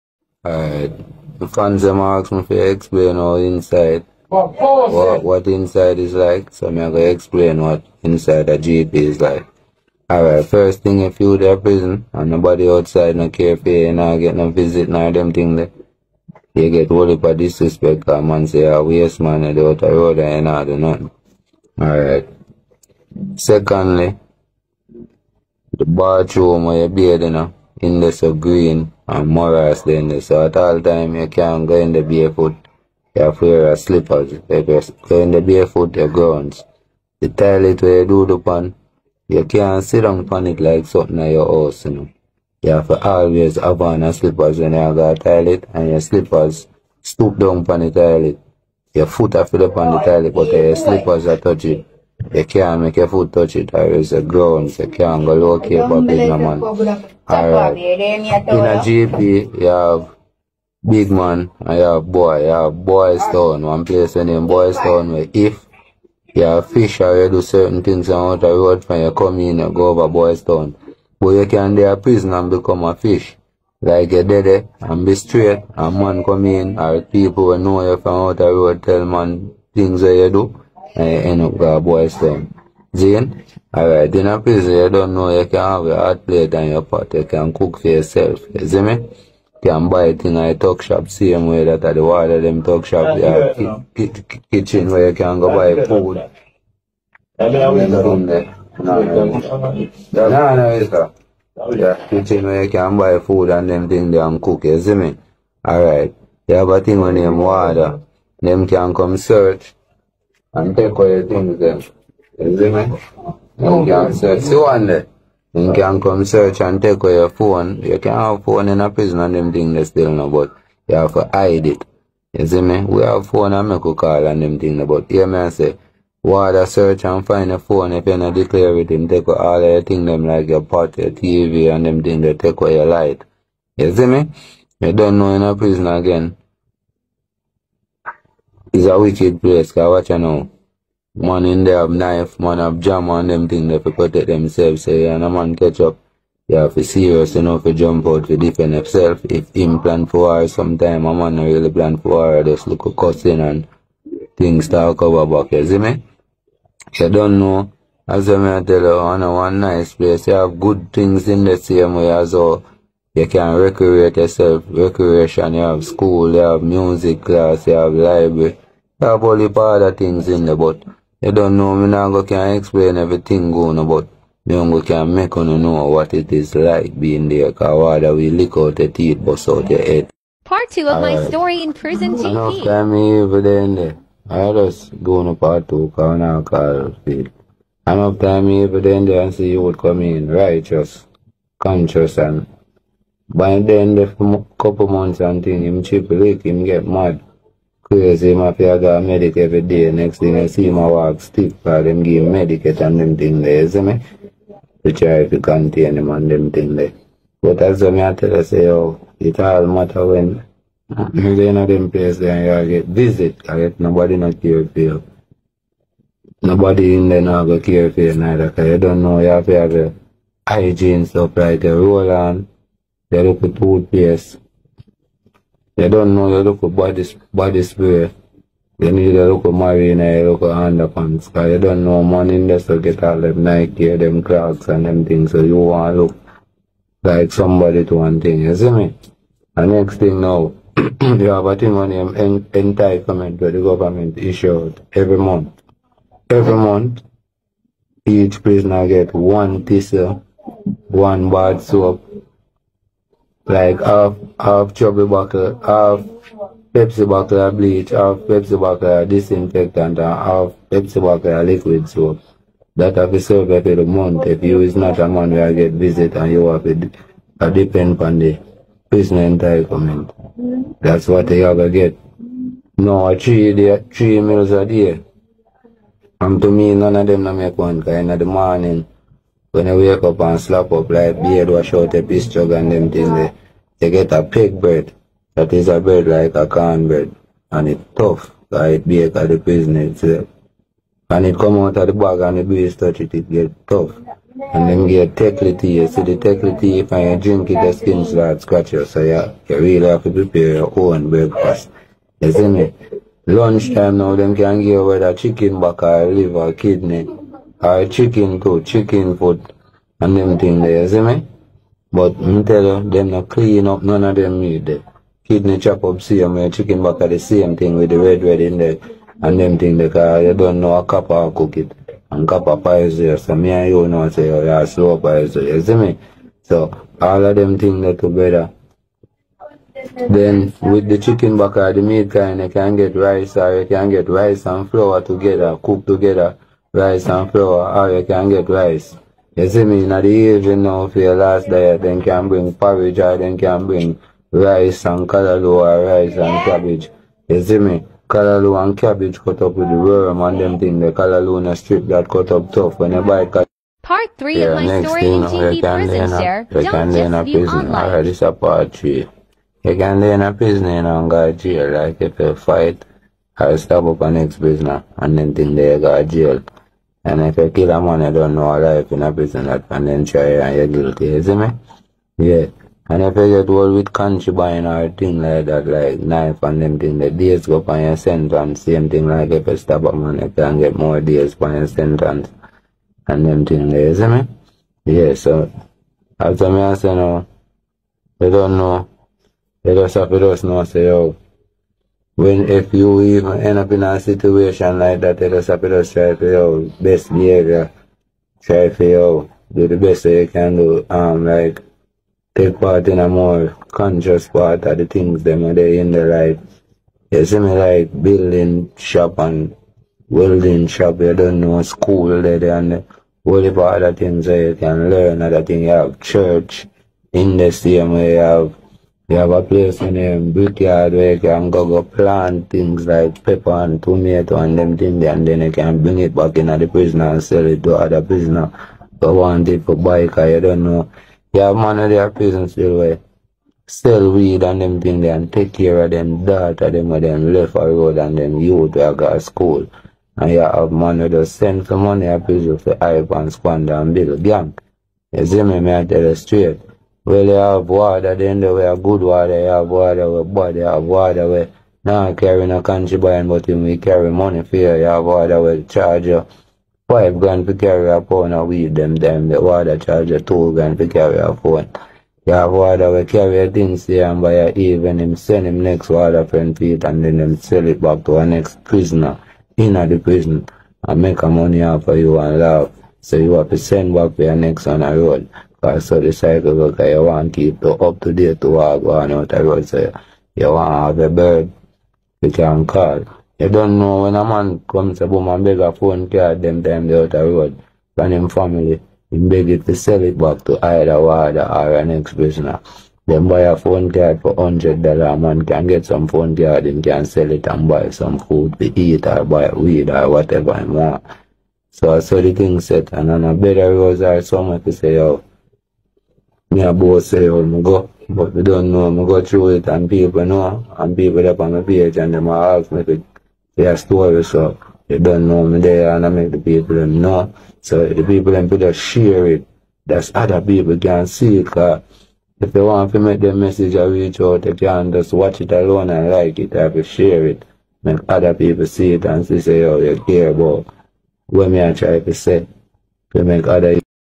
The Real G.P E❌PERℹENCE *Must Watch*
Ex Convict Gives Insight On Past Experience At General Penetentiary (GP) *Must Watch*
Follow me on other social media platforms and stay up to date: